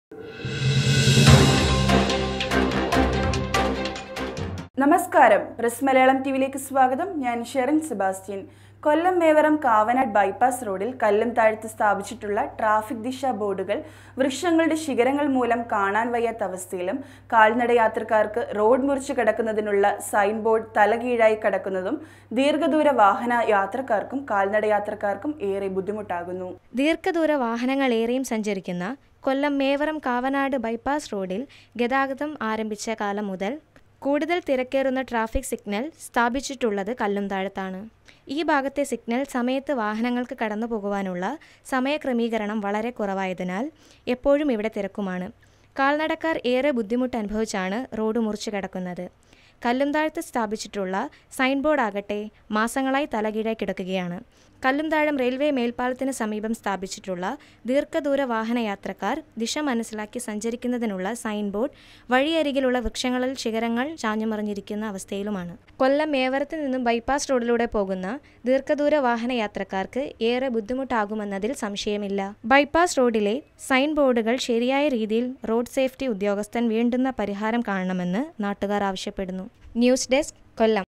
കൊല്ലം മേവറം കാവനാട് Bypass റോഡിൽ ദിശാ ബോർഡുകൾ വൃക്ഷങ്ങളുടെ ശിഖരങ്ങൾ മൂലം കാണാൻ വയ്യാത്ത അവസ്ഥയിൽ.
കൊല്ലം മേവറം കാവനാട് ബൈപാസ്സ് റോഡിൽ കല്ലുംതാഴത്തു  സ്ഥാപിച്ചിട്ടുള്ള ട്രാഫിക് ദിശാ ബോർഡുകൾ വൃക്ഷങ്ങളുടെ ശിഖരങ്ങൾ  മൂലം കാണാൻ വയ്യാത്ത അവസ്ഥയിലും, കാൽനട യാത്രക്കാർക്കു  റോഡ് മുറിച്ചു കടക്കുന്നതിനുള്ള സൈൻ ബോർഡ് തല കീഴായി കിടക്കുന്നതും ദീർഘ ദൂര വാഹന യാത്രക്കാർക്കും, കാൽ നട യാത്രക്കാർക്കും  ഏറെ ബുദ്ധിമുട്ട് ആകുന്നു./pressmalayalamTv